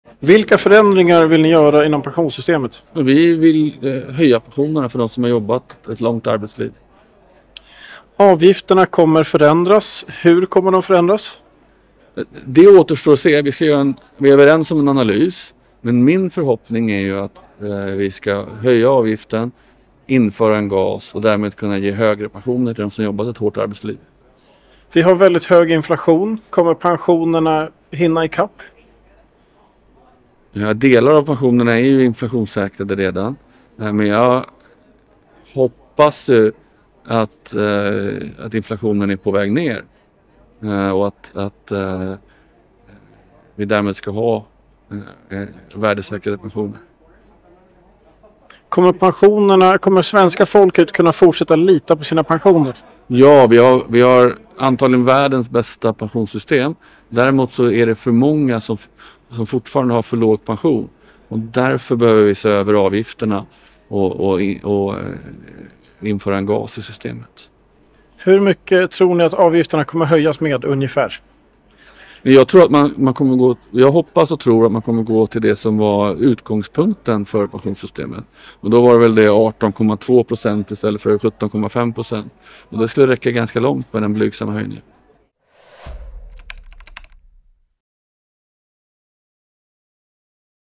Anders Ygeman (S): ”Vi har antagligen världens bästa pensionssystem”
Inflationen har drabbat Sverige hårt och pensionerna har inte följt med prishöjningarna. Nu tillsätter riksdagens pensionsgrupp två utredningar för att se över pensionerna.

Äntligen, en svensk oberoende nyhetstidning. Fri från politisk vinkling – fri från reklam. Testa nu för endast 1 krona här